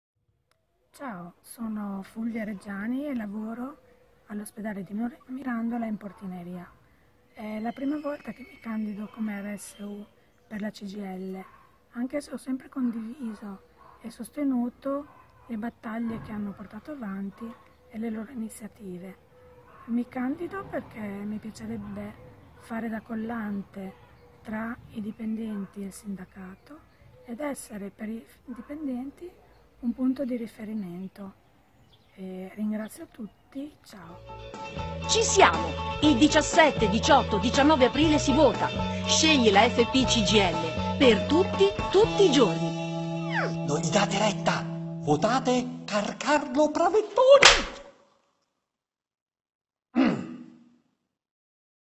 Fulvia Reggiani candidata RSU 2018 Fp Cgil Modena
Fulvia Reggiani, operatrice della portineria dell'ospedale di Mirandola, spiega perché si è candidata Rsu Fp Cgil
Per tutti. Tutti i giorni. Siamo Valore Pubblico